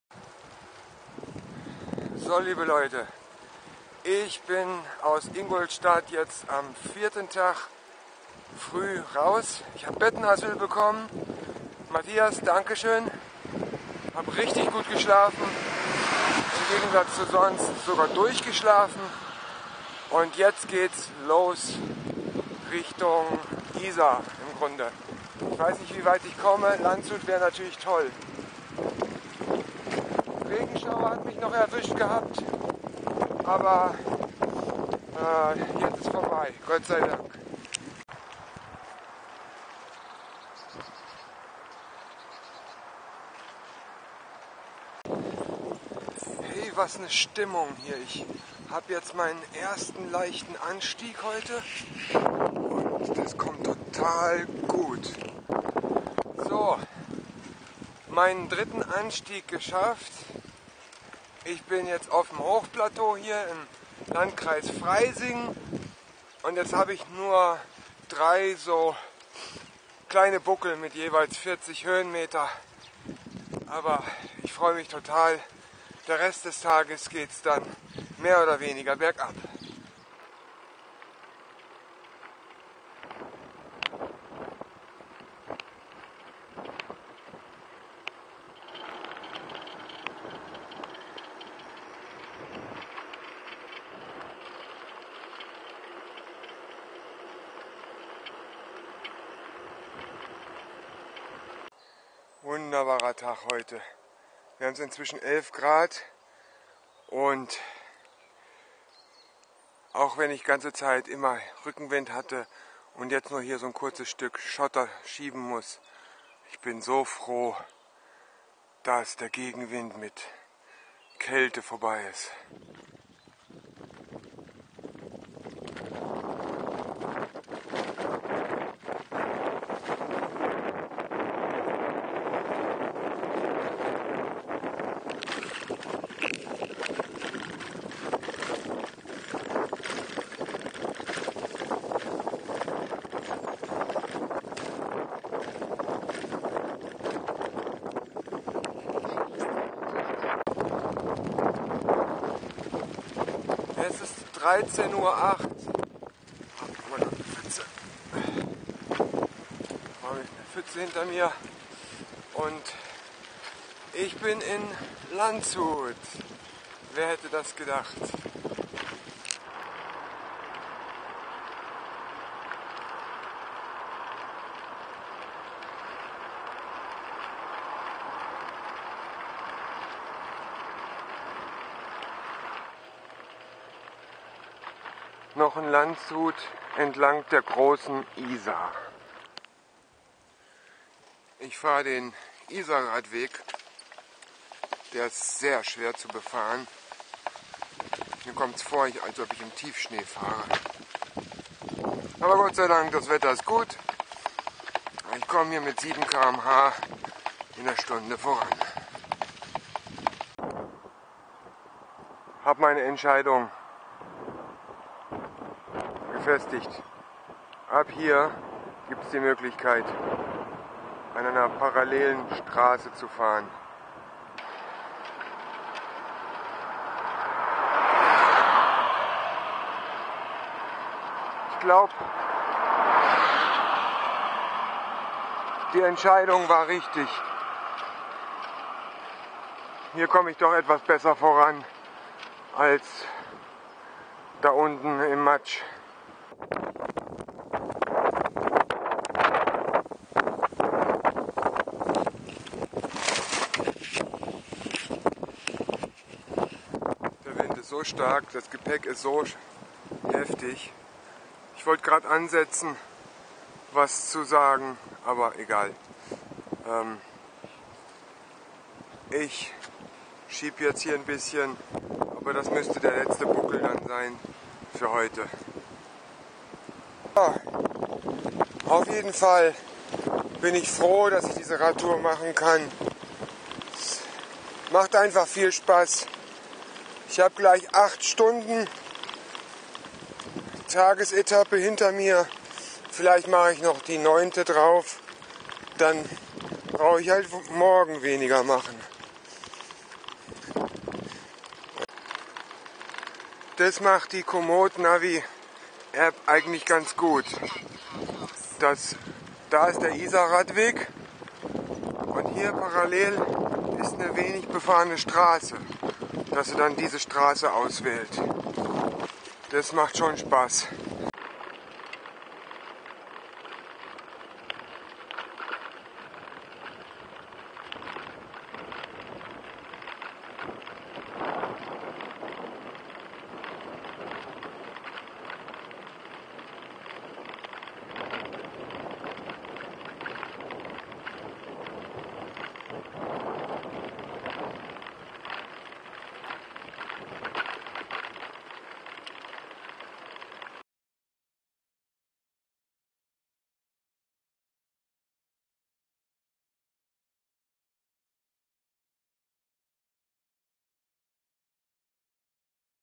OmaRadTour 4. Tag: Von Ingolstadt nach Mamming / Isar (Isarradweg)
Motiviert und durch Bettenasyl gut ausgeruht geht es in den vierten Tag: Diese Tagesetappe war durchwachsen mit unglaublich schönen Wolkenstimmungen und wunderschönen Abschnitten.